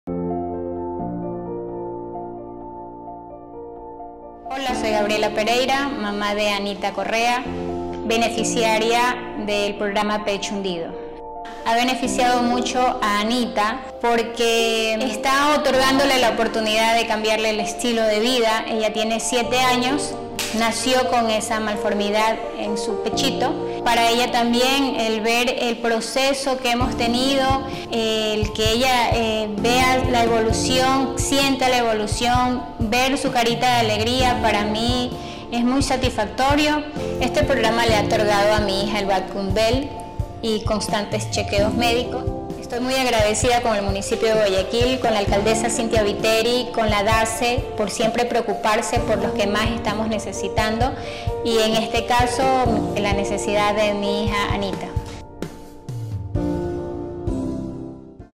¡Beneficiaria Pecho Hundido!
Anita es una niña de 7 años que nació con una malformación en su pecho.

El programa Pecho Hundido beneficia a pacientes que no cuentan con recursos económicos y que se encuentran en estas condiciones, brindándoles citas médicas con especialistas y acceso al aparato Vacuum Bell que será de gran ayuda durante el tratamiento.

¡Anita y su mamá Gabriela no están solas, trabajamos día a día por su bienestar! 💙